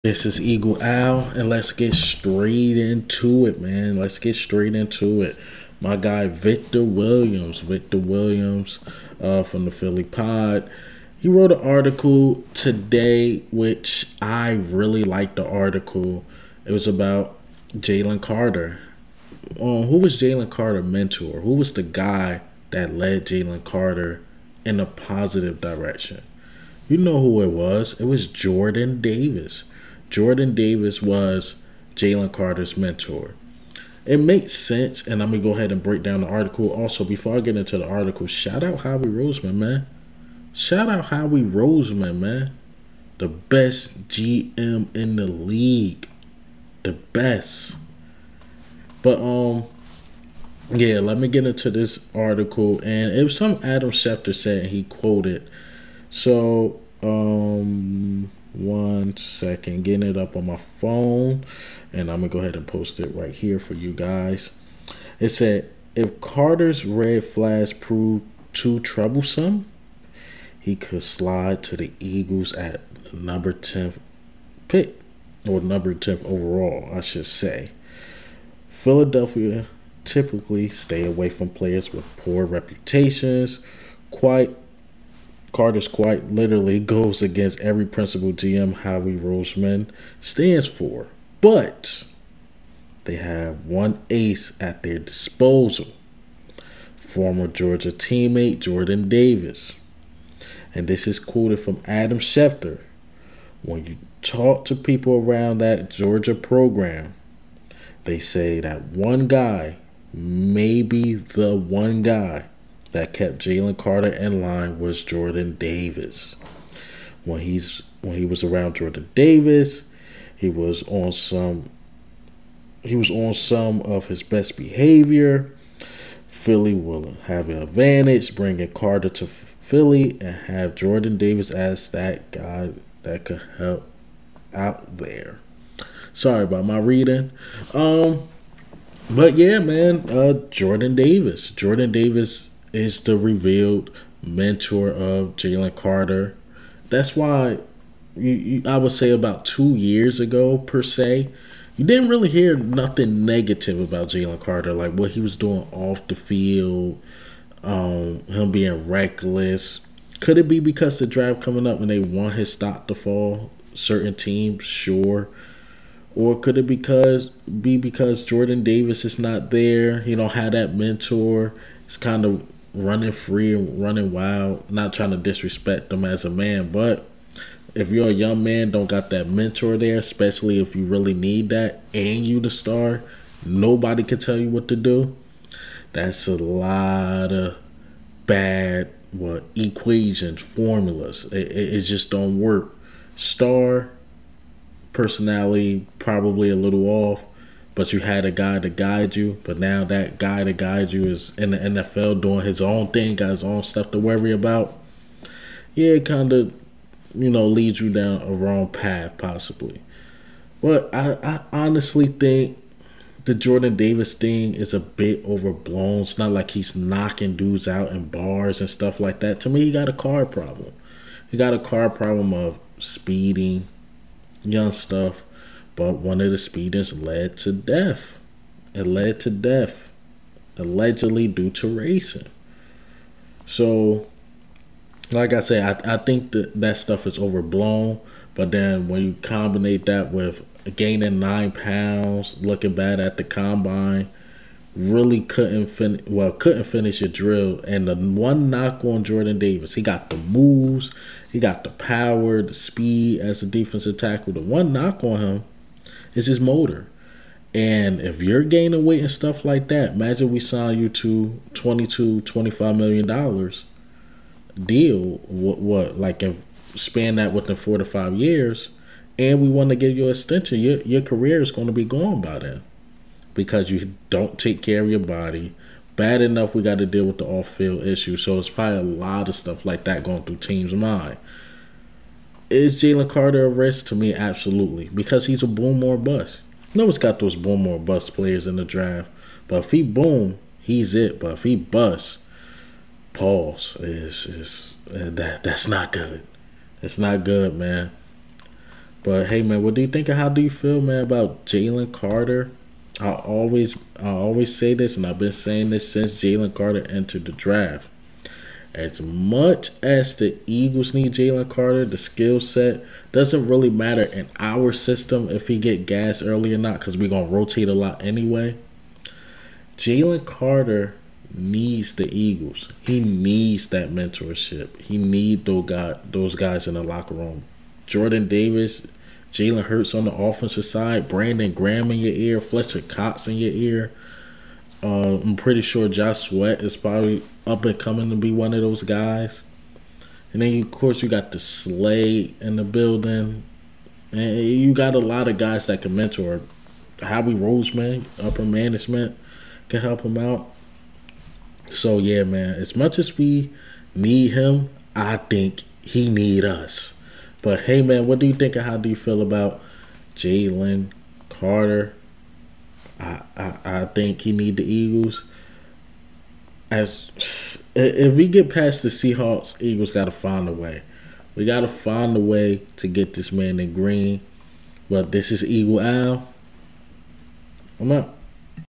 Jalen Carter's Mentor Revealed! How Eagles Jordan Davis Keeps Him on Track!
In this exclusive reveal, we uncover the mystery behind Jalen Carter's College Football success! Join us as we unveil the influential mentor who has been instrumental in keeping Jalen Carter on track to becoming a football star. And the surprising name is none other than Eagles' Jordan Davis! Don't miss out on this inside scoop as we delve into the special relationship between Jalen Carter and Eagles' Jordan Davis, and how it has shaped Carter's College career. Tune in now to learn the secret behind Jalen Carter's meteoric rise in the world of football!